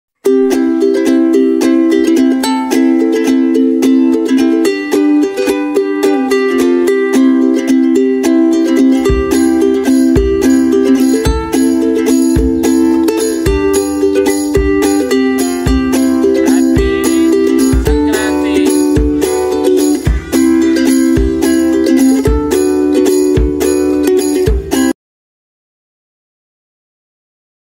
Celebration
సంక్రాంతి ముగ్గులు వేస్తున్న పిల్లలూ